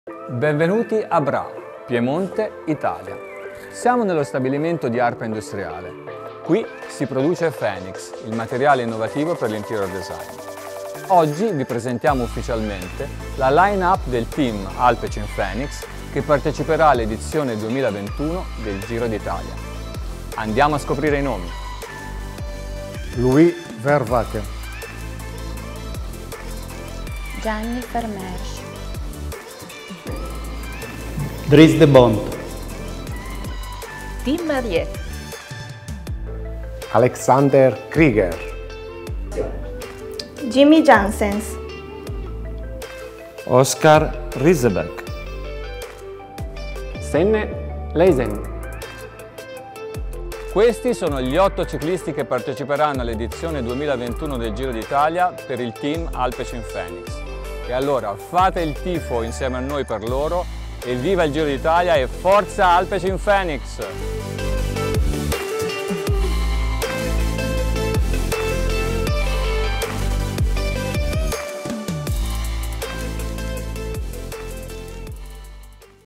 Alpecin-Fenix lineup for the 2021 Giro d'Italia
Our first ever Grand Tour deserves a unique lineup presentation, right? It's perfect occasion for our Italian partner FENIX to help us out, as the 2021 Giro d'Italia kicks off in their home region, Piedmont.

The FENIX staff presents you our 8 AlpecinFenix riders for the 2021 Giro!